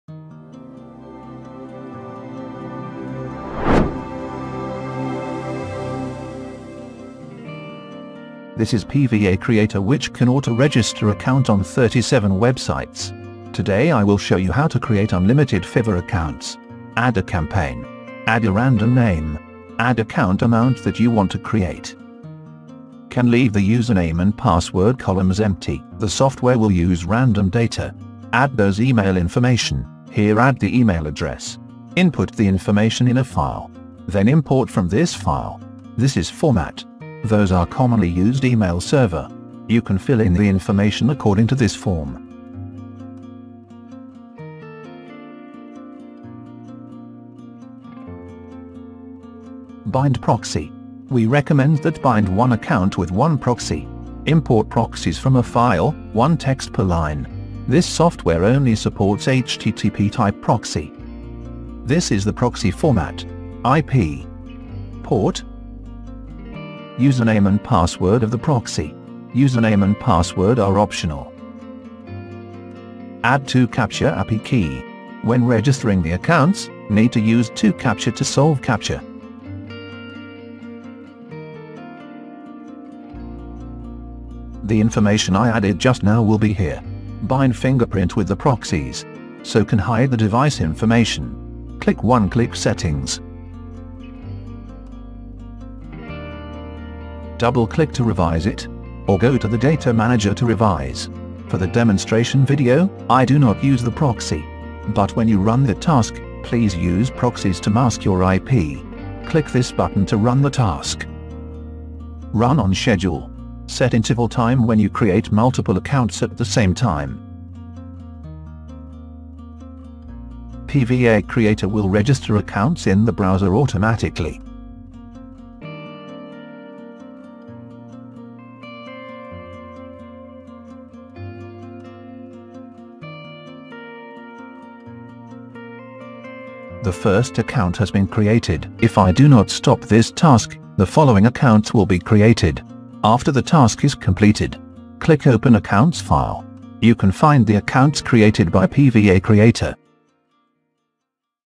How to Create Unlimited Fiverr accounts|PVA Creator
PVA Creator can auto-register unlimited accounts on 37 websites, we are adding more.
If you do no find the site, please submit your request